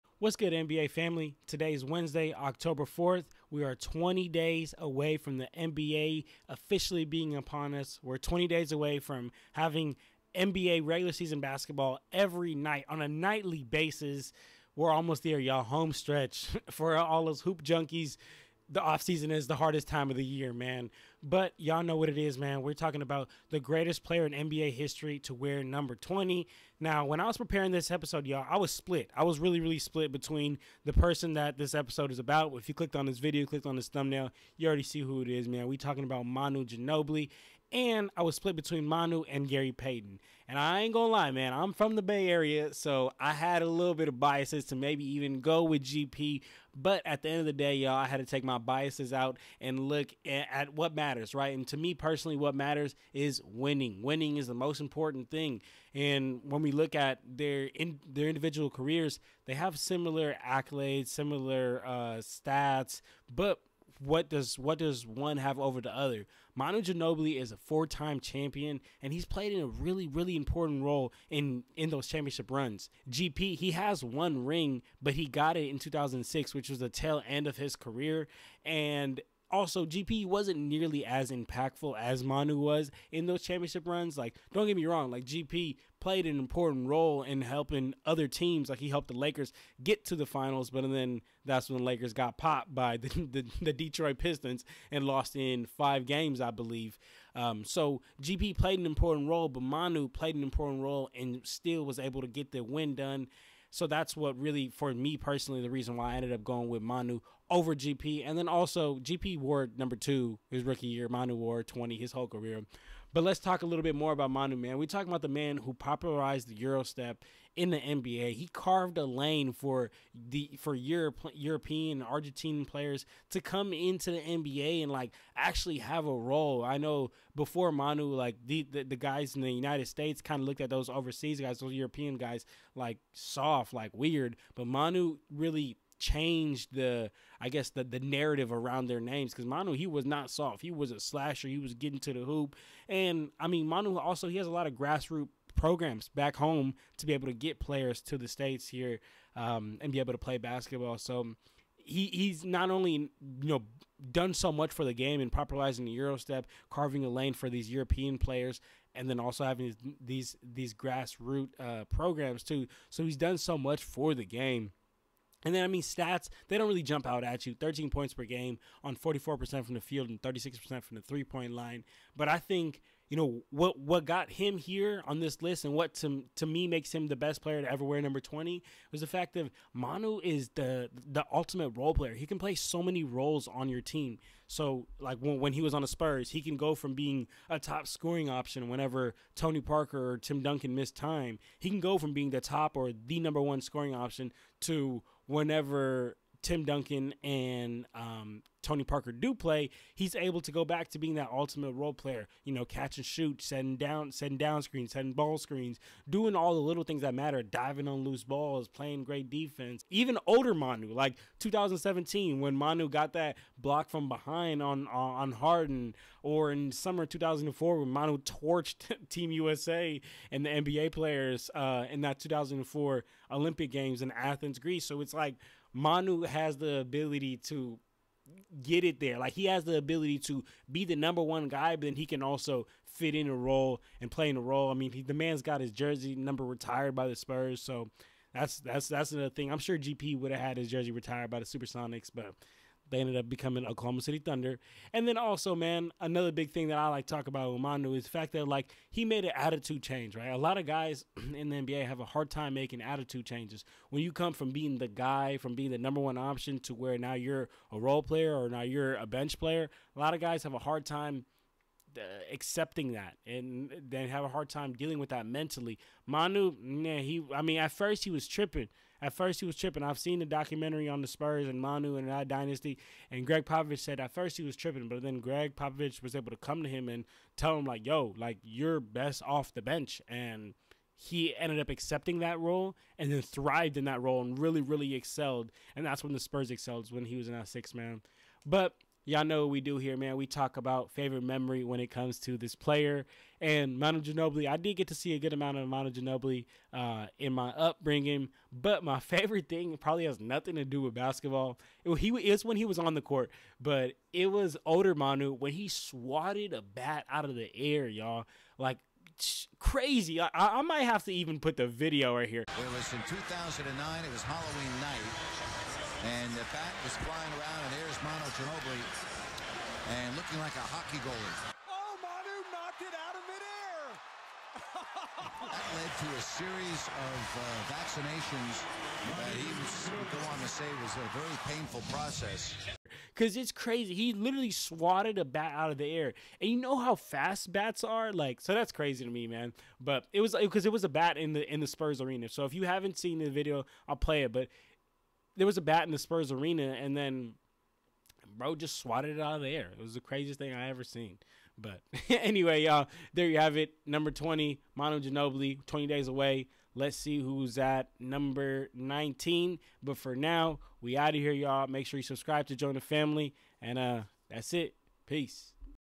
Countdown to NBA Season: 20 Days Left | Manu Ginobili
20 DAYS LEFT UNTIL NBA REGUAR SEASON.
Lets talk about Manu Ginobili